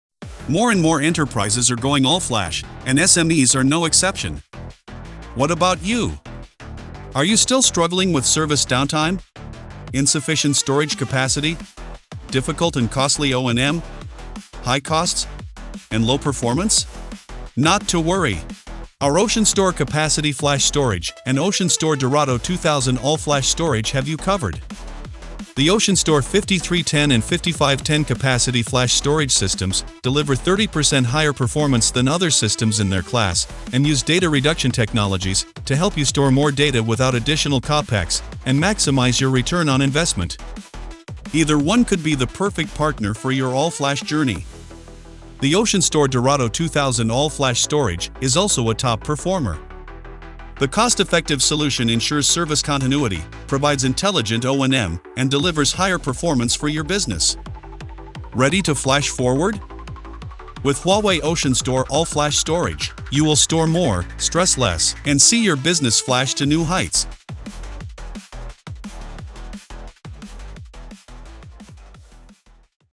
AllFlash4All-Flash to New Heights with Huawei OceanStor All-Flash Storage
Store more, stress less! 👀 Huawei OceanStor all-flash storage provides high capacity and performance to fit the diverse data needs of SMEs. Discover how HuaweiStorage